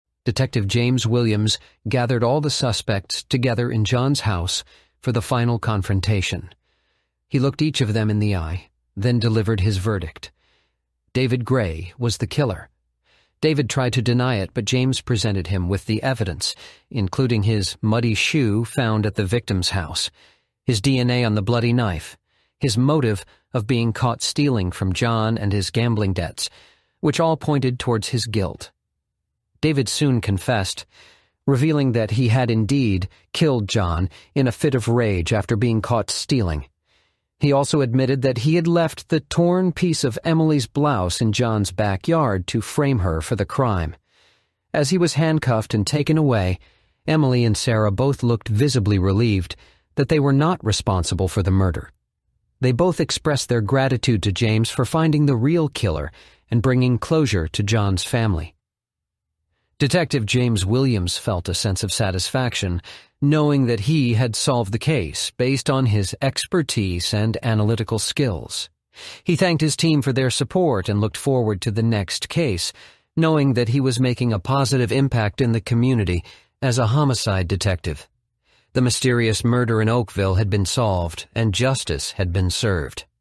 The Mysterious Murder in Oakville | Part 5
Investigate with Detective James Williams about the Mysterious murder of a retired Banker in a quiet suburban town, Oakville.